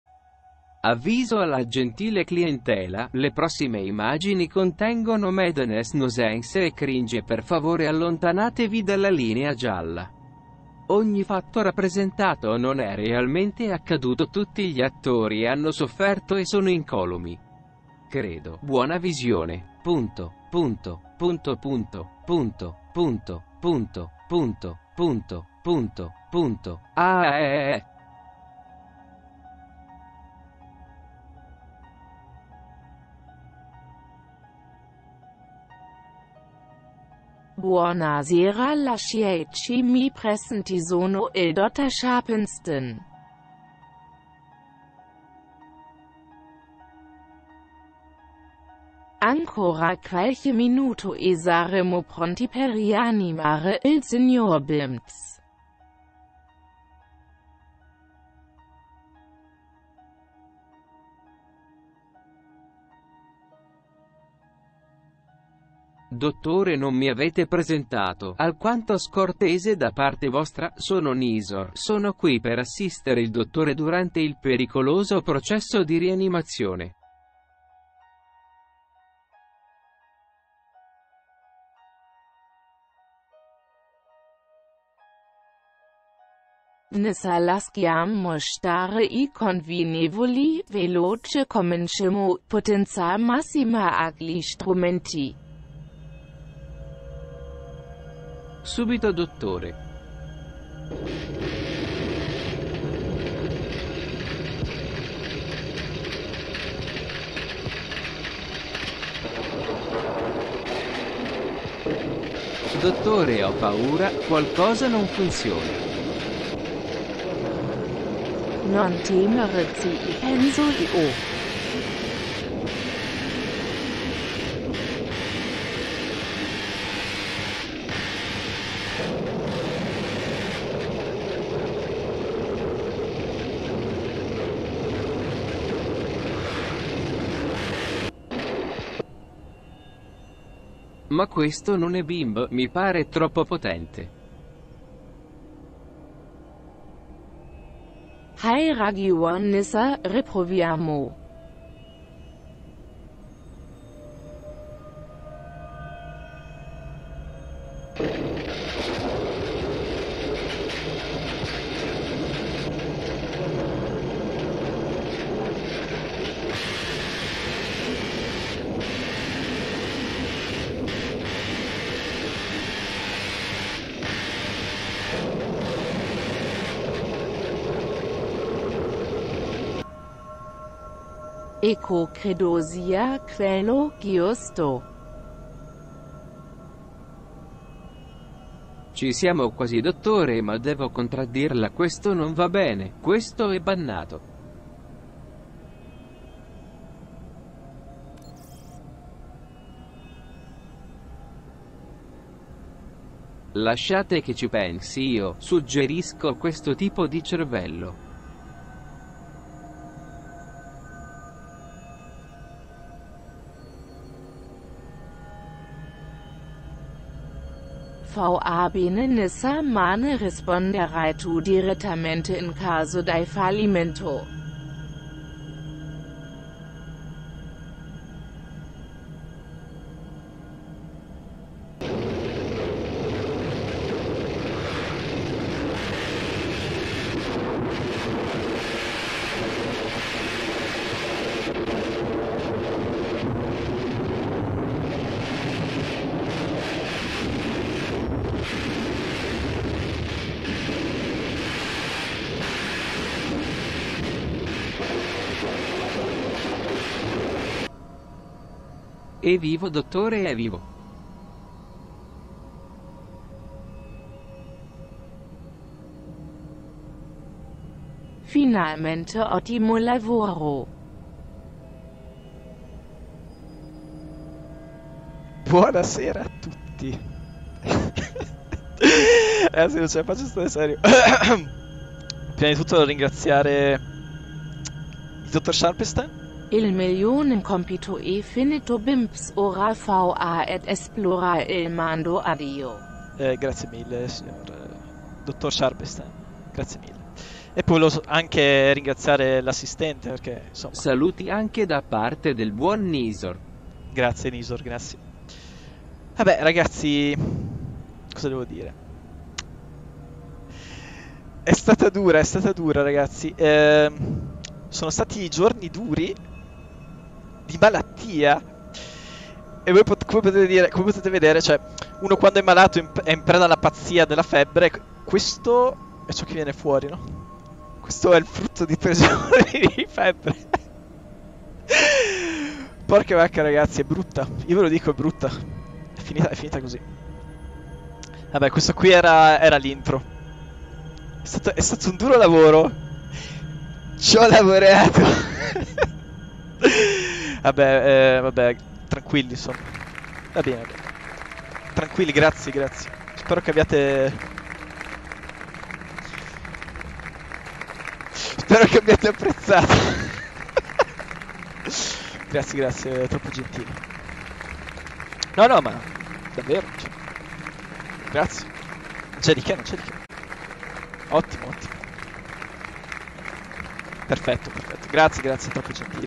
Riesumazione: La Madness [intro twitch 29/01/19]
Dopo svariati giorni passati sotto una febbre insana, ho deciso di fare il gran comeback su twitch.

per la partecipazione(forzata).